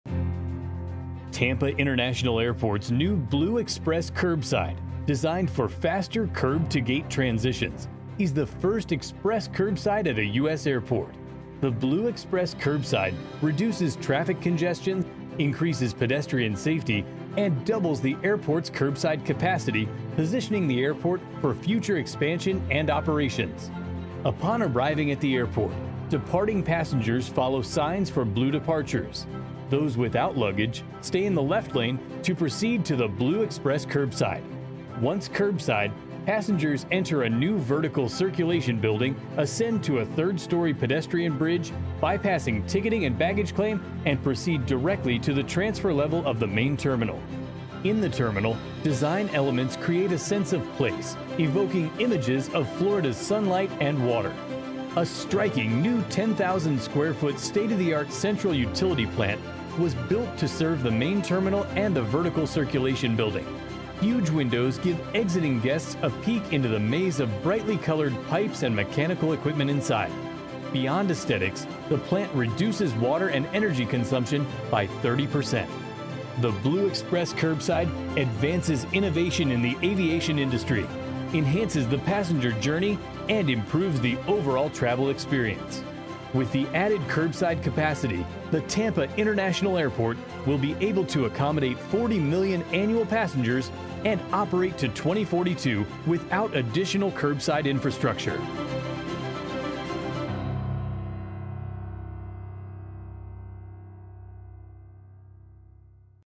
Tampa International Airport Express Curbside | HNTB
HNTB is serving as architect and engineer of record for a progressive design-build project team, providing an innovative solution for the $360M curbside expansion, central plant, and APM Station project at Tampa International Airport. HNTB developed the original master plan, designed the world’s first “Express Curbside” for passengers with carry-on only bags. This allows passengers who are not checking bags with airlines to access an express curb that by-passes the ticket lobby entirely and provides direct access to gates. The innovation reduces curbside congestion while providing a new enhanced level of customer service.

The project creates four curbside lanes stacked on two levels, at the dual Blue and Red sides of the main terminal for a total of 16 new curbside lanes with two vertical circulation buildings. The project also includes demolition of existing buildings, a sustainable Central Utility Plant, an APM station for Airside D, a pedestrian bridge, airport loading and receiving docks, elevated approach and exit roadway bridges, and an airport office building in the Long-Term Parking Garage.